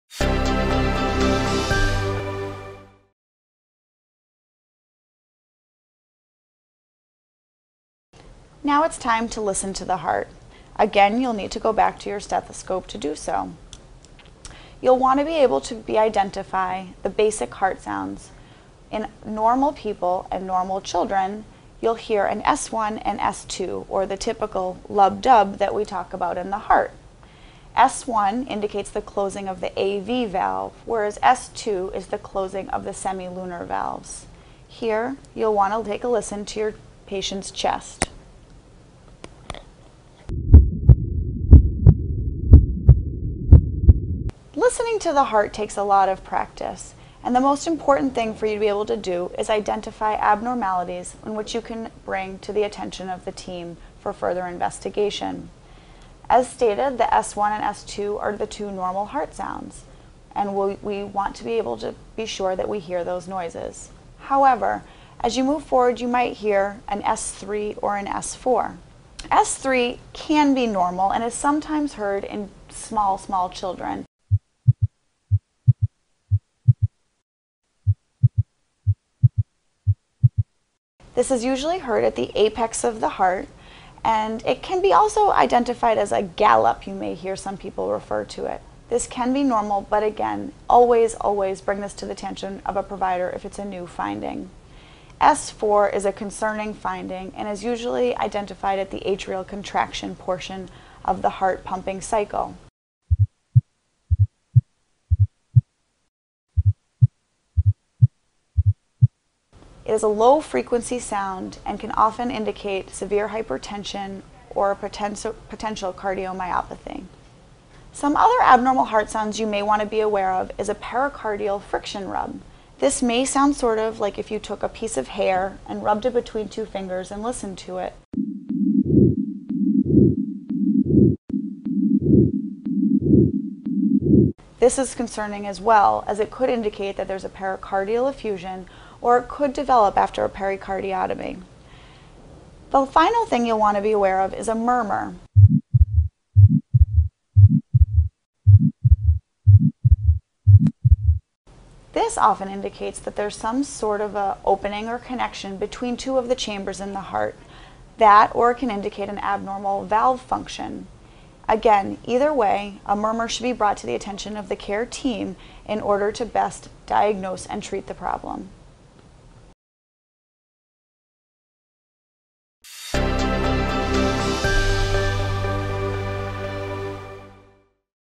Cardiovascular Assessment: Auscultation by B. Leary | OPENPediatrics
Quick Concepts are short videos that describe a key physiological or theoretical concept or demonstrate a brief procedure. In this video, Brienne Leary reviews normal and abnormal heart sounds you may hear when listening to your patient’s heart.

AUTHOR
Brienne Leary, RN, BSN, CPON, CCRN
Staff Nurse II
Medical-Surgical Intensive Care Unit
Nursing Science Research Fellow
Boston Children's Hospital

DATES
Initial Publication: February 13, 2024

CITATION
Leary B, Wolbrink TA, Imprescia A, Beke D. Cardiovascular Assessment: Auscultation. 02/2024. From: Leary B, Wolbrink TA, Imprescia A, Beke D. Cardiovascular Assessment. 6/2015. OPENPediatrics.